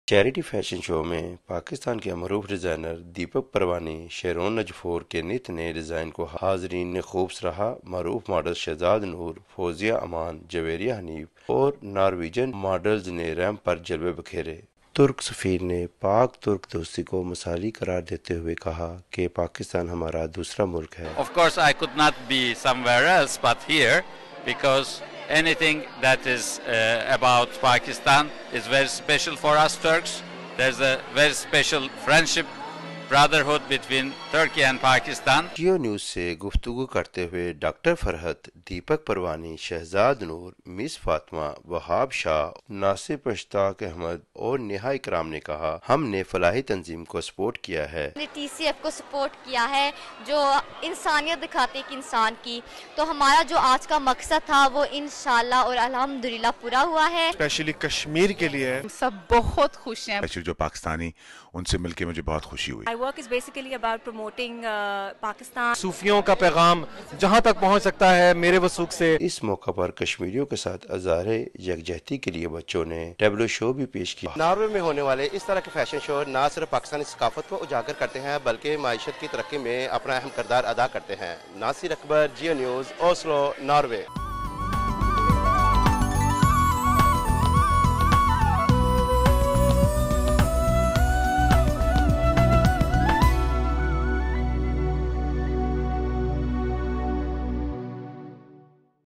Designer Deepak Perwani Showcases New Collection At Oslo’s Charity Fashion Show For Kashmir
Geo News Special – Pakistan’s Designer Deepak Perwani Showcases New Collection At Oslo’s Charity Fashion Show For Kashmir